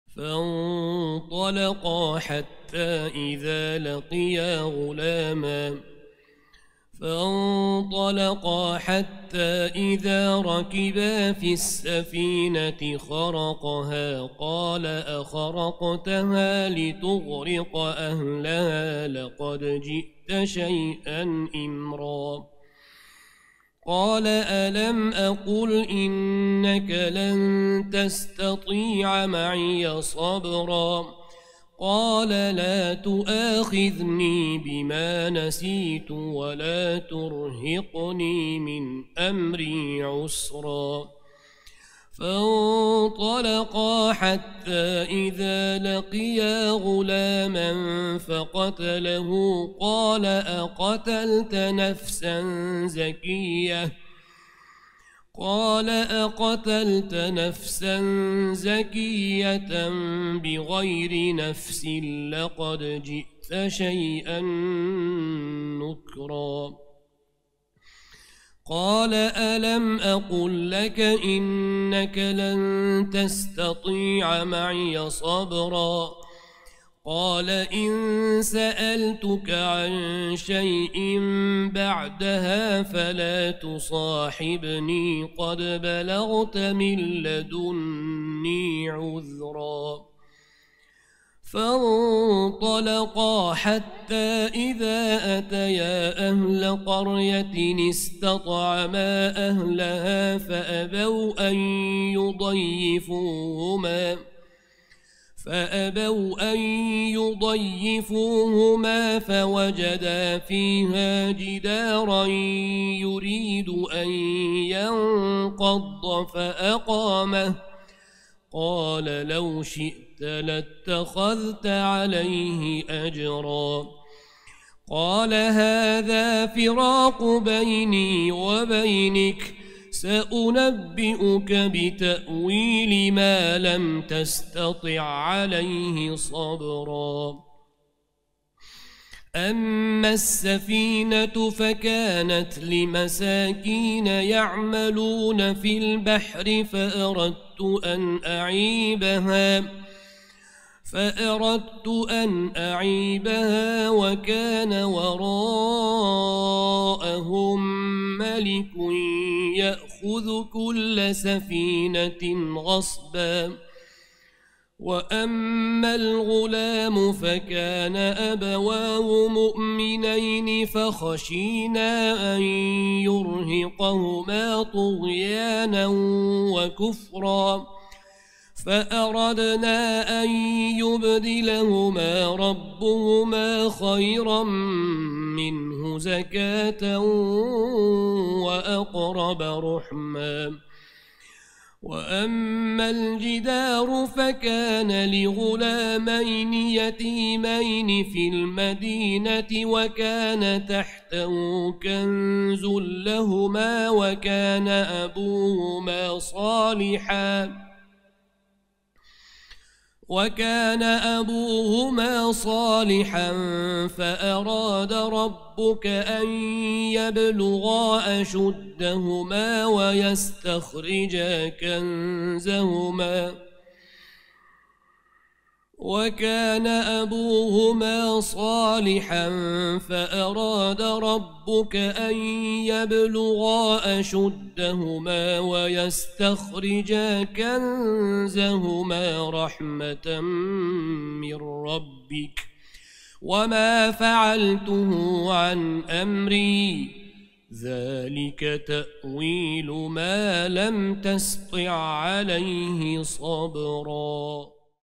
Kahf surasi 71-oyatdan 82-oyatgacha | Hasanxon Yahyo Abdulmajid | Taroveh namozi 12-kun
Shayx Muhammad Sodiq Muhammad Yusuf» jome' masjidi imom noibi Hasanxon Yahyo Abdulmajid

Ramazon 1444 | 2023